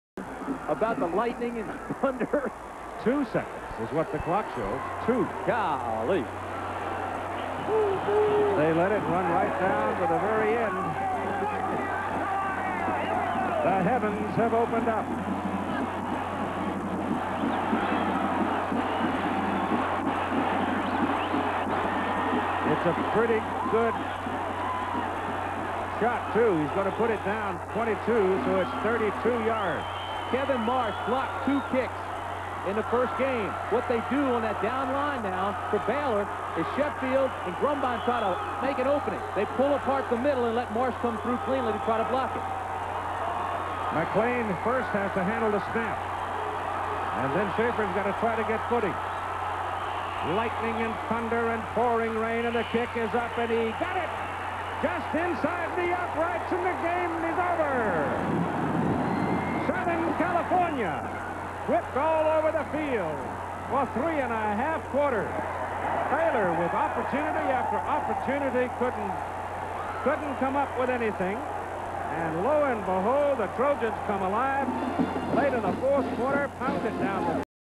USC Trojans vs Baylor Bears (September 20, 1986) "The Trojans Beat The Bears In A Thunderstorm"
(Time: 1:18)  Keith Jackson / Tim Brant  (ABC Sports)